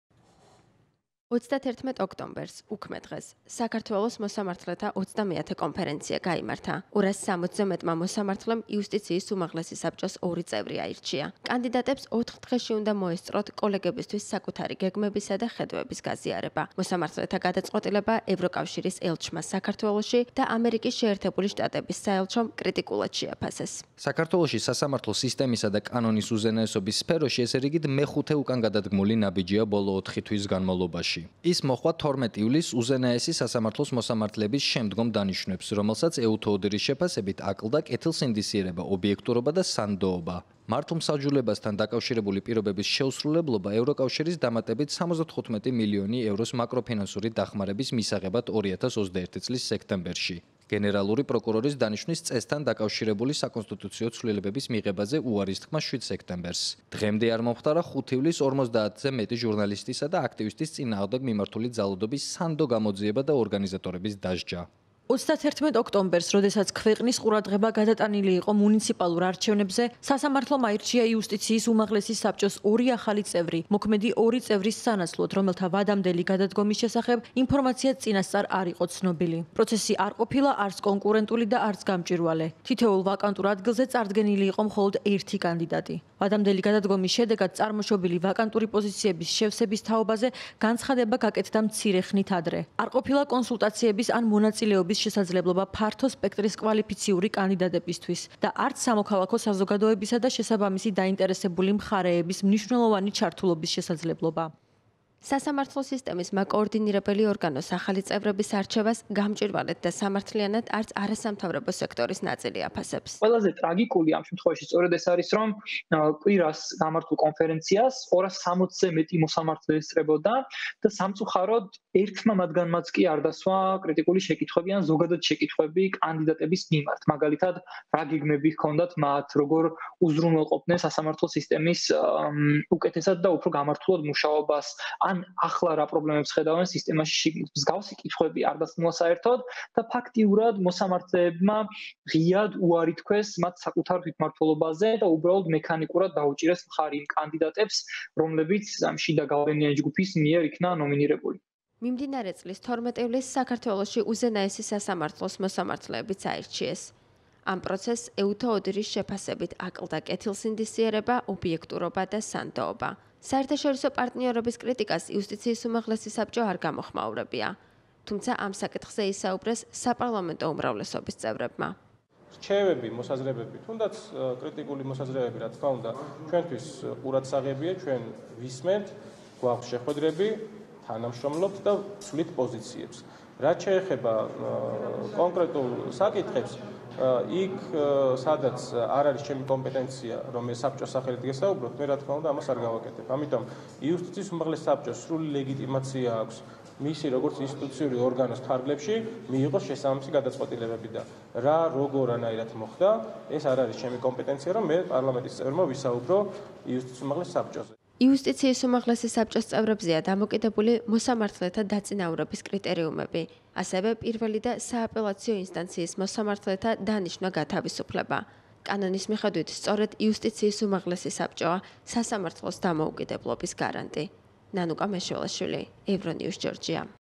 იუსტიციის უმაღლესი საბჭოს წევრების დანიშვნას კრიტკულად აფასებენ
იუსტიციის უმაღლესი საბჭოს წევრებზეა დამოკიდებული მოსამართლეთა დაწინაურების კრიტერიუმები. ასევე, პირველი და სააპელაციო ინსტანციის მოსამართლეთა დანიშვნა-გათავისუფლების საკითხი. კანონის მიხედვით, სწორედ იუსტიციის უმაღლესი საბჭოა სასამართლოს დამოუკიდებლობის გარანტი